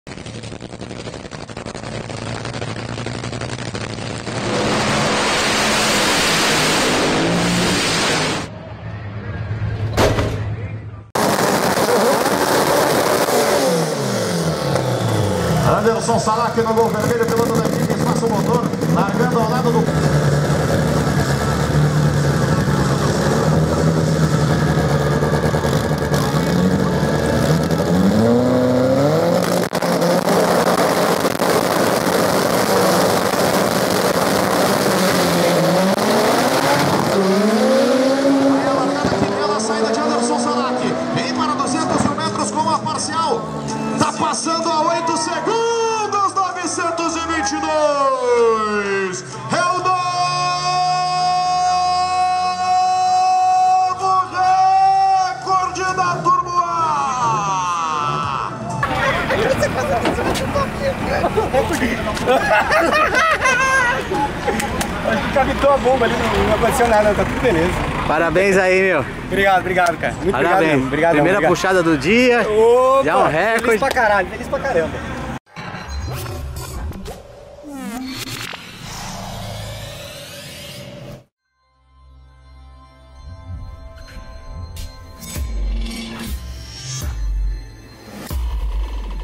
Gol DT-A - Anderson Salak - Recorde no Velopark
Anderson Salak, da equipe Espaço Motor, acelerou o Gol AP 16V turbo e entrou na casa dos 8 segundos, baixando o recorde da categoria com 8s922.